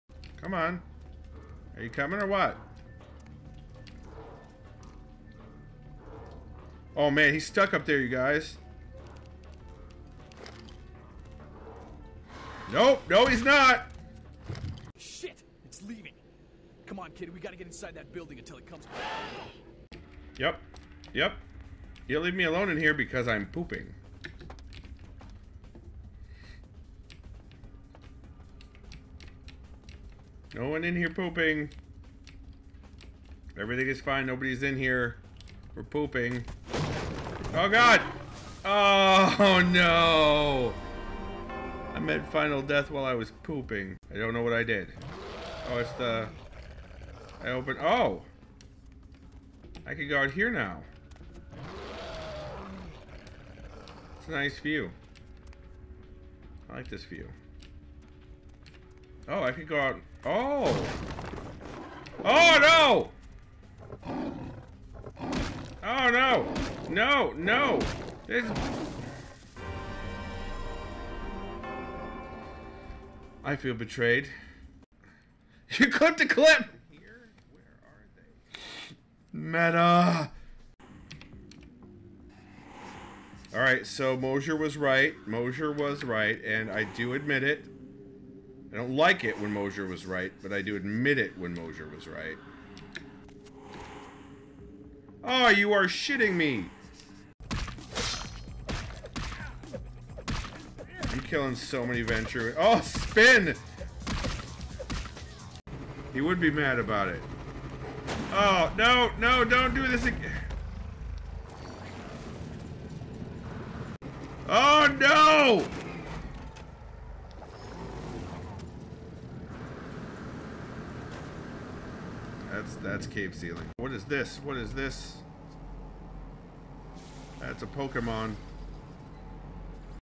Skippy Clips! Part 10
Here's a collection of ten clips from Skippy's earlier streams, circa 2019!

Tune in for live Twitch streams on Tuesday, Thursdays, and on the weekend

Everywhere else Skippy is!